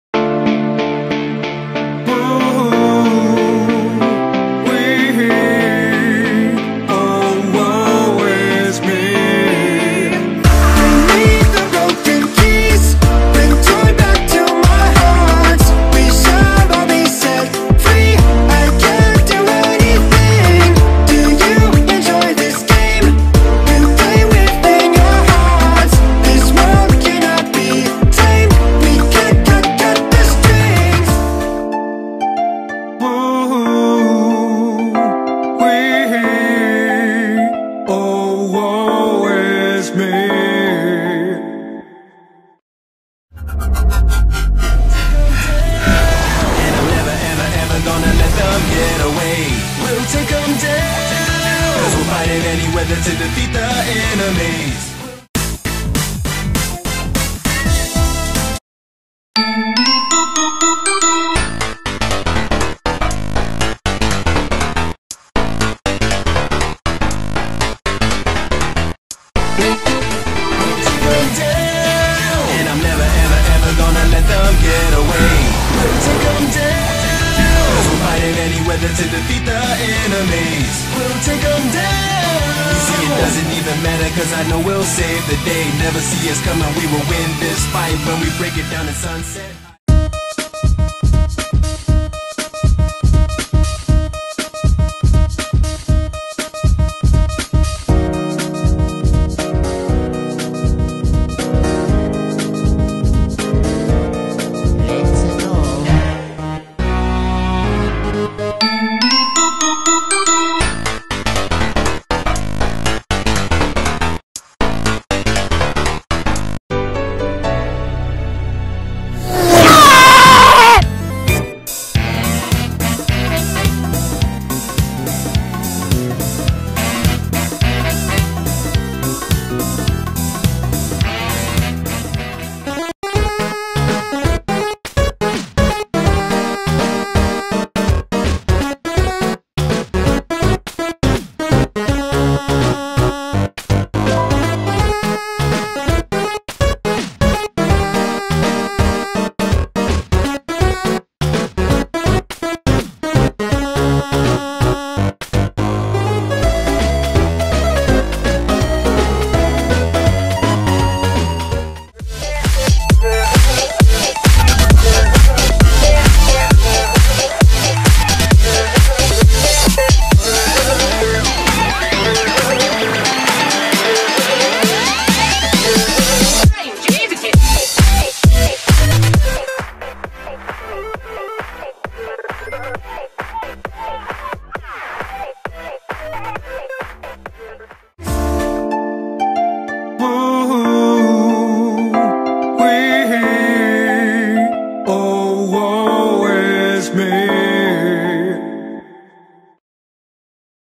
Broken Keys, Chapter 3 (Acorn Trouble)
BJ finds a key (in the rain.cally dimension) into world one which leads them into Acorn Plains, where they meet up with the first bad guy, Lancer